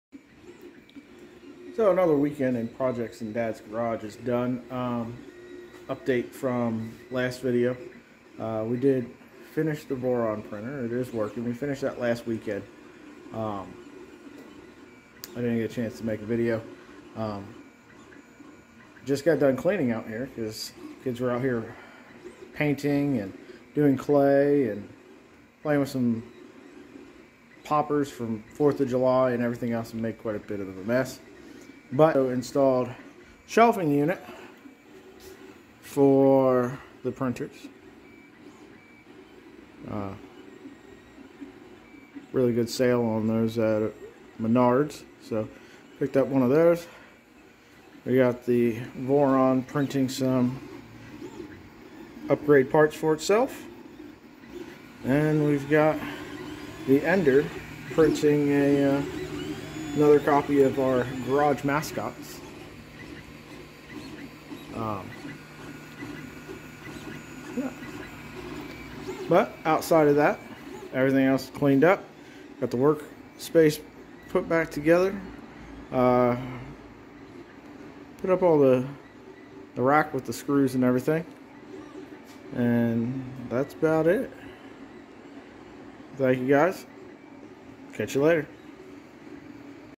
Voron 2.4r2 and Garage update.
Finished the Voron 2.4r2 last weekend. installed shelf and cleaned up after a long weekend.